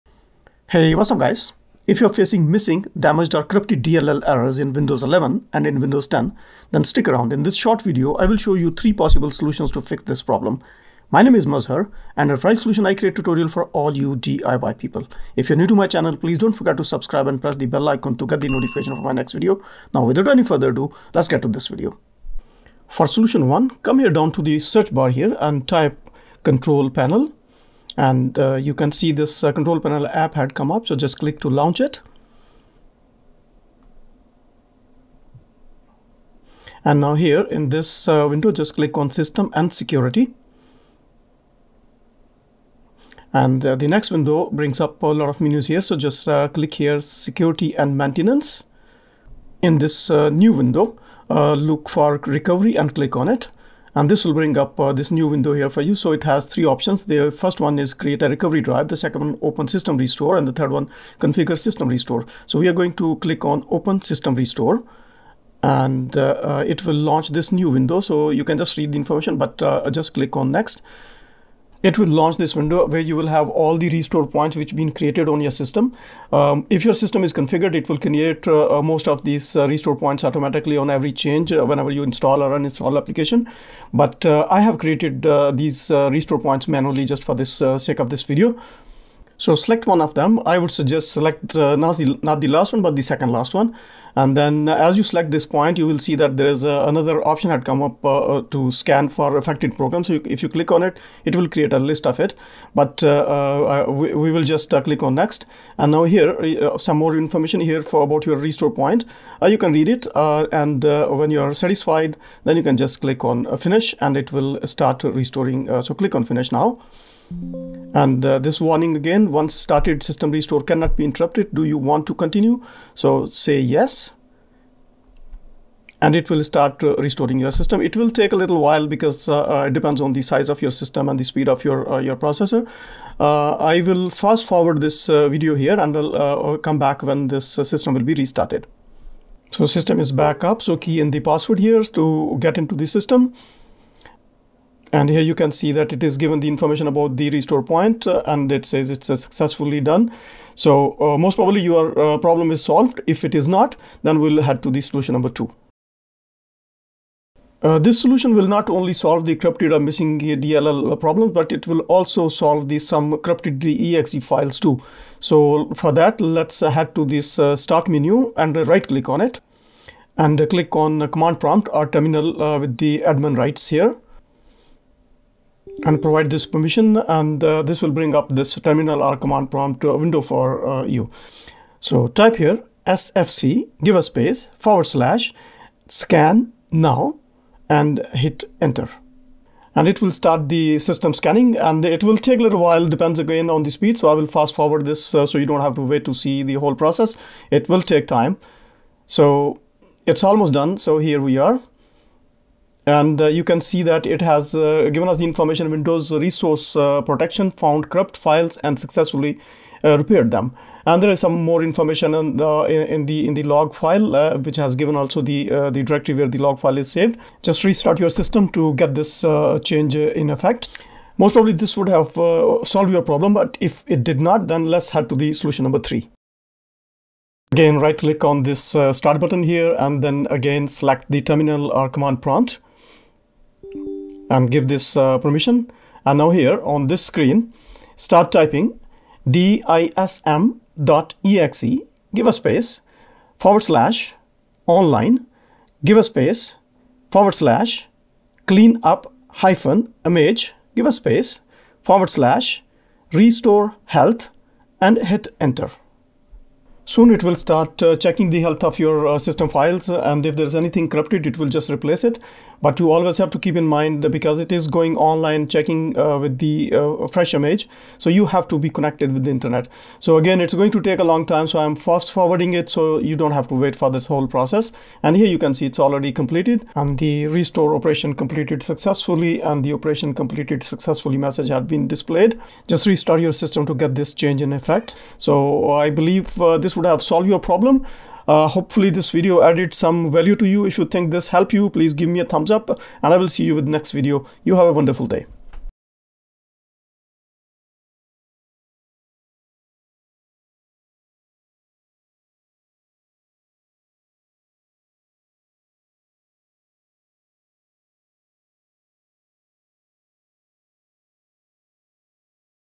How to Fix DLL Errors in Windows 11 | Fix All missing or Corrupted DLL errors in Windows 11 - 10
How to Fix DLL Errors in Windows 11 | Fix All missing or Corrupted DLL errors in Windows 11 - 10. In this video I will show you how to Fix All DLL not found, DLL missing or Corrupted DLL errors in Windows 11 and in Windows 10 step by step.

*Click on the timestamps to directly jump to the chapter of your interest*
00:00 Introduction
00:23 Solution 1
02:36 Solution 2
03:43 Solution 3
04:45 Thanks for Watching - Subscribe

How to Enable Missing Security Tab in Folder Properties in Windows 11 and in Windows 10